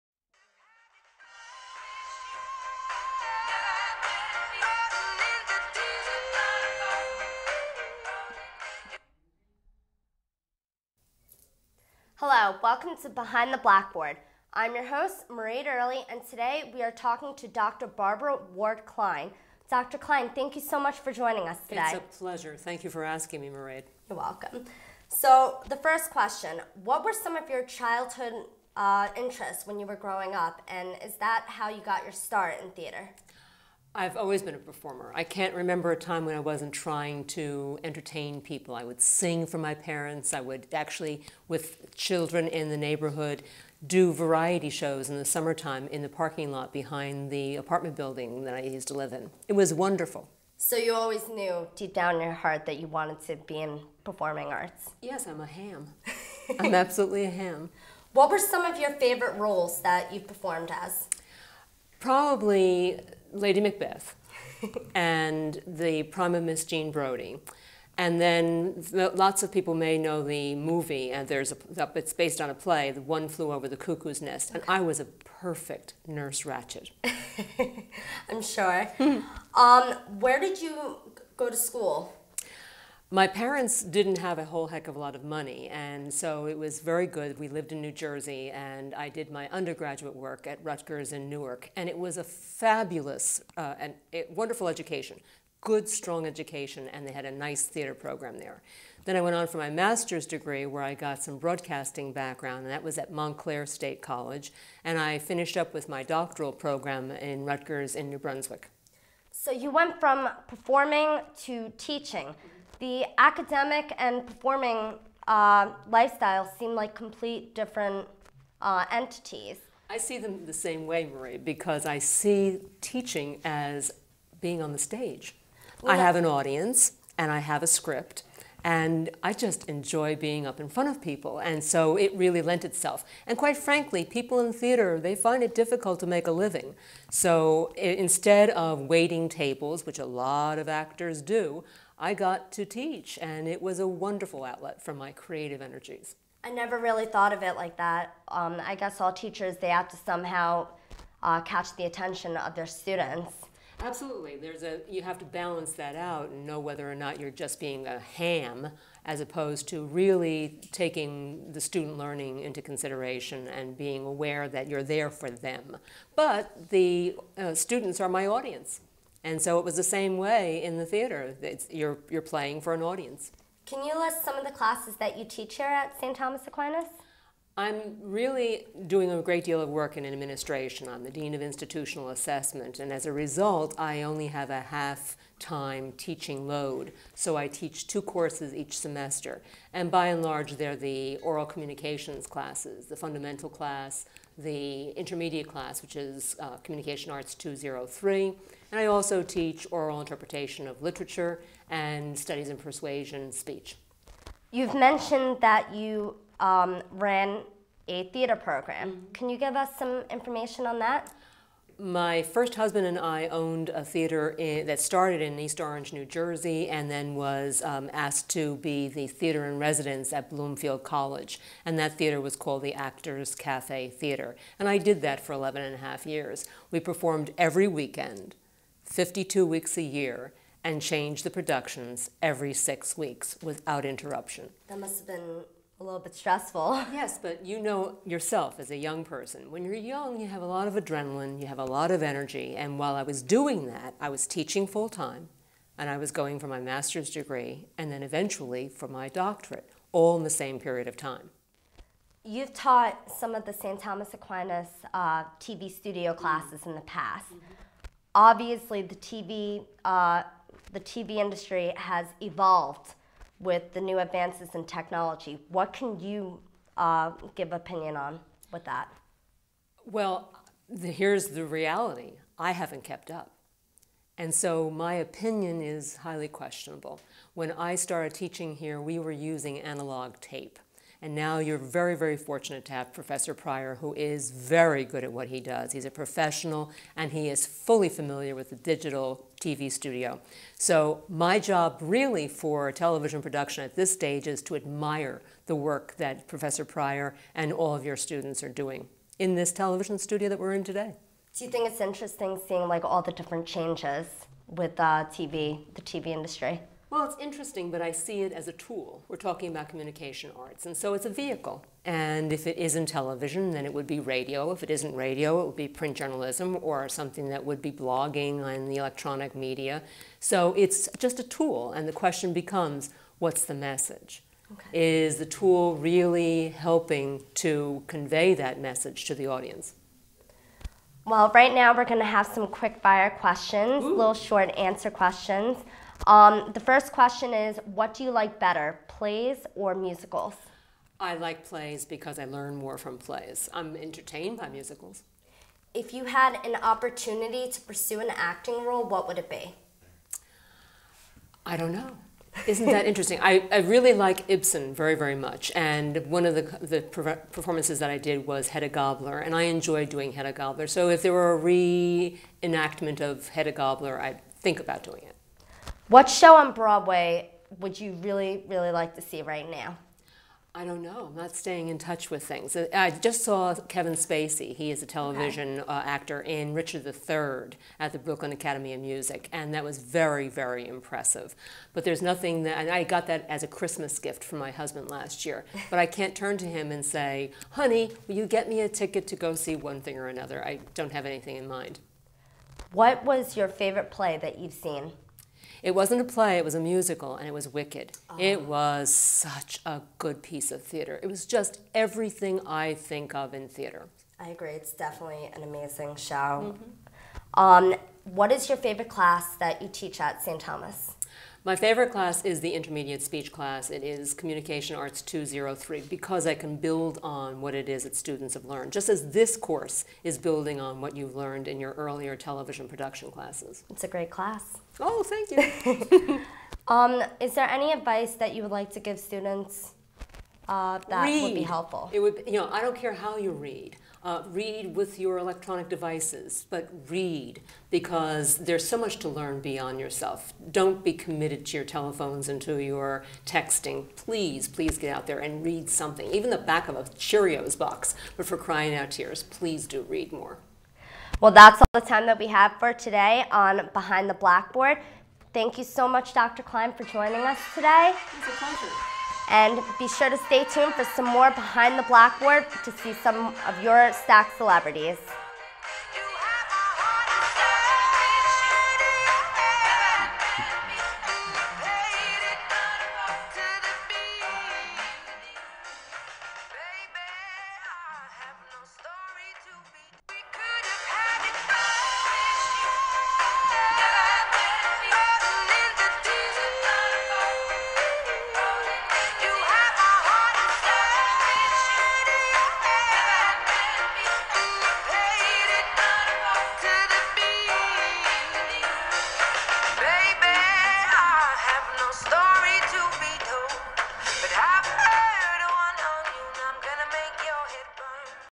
Behind the Blackboard, Dr. Barbara Ward Klein
Interview #2
Video Magazine class interviews another of our celebrity Communication Arts Faculty at St. Thomas Aquinas College in Sparkill,NY.
Dr. Klein is interviewed by Mairead Early.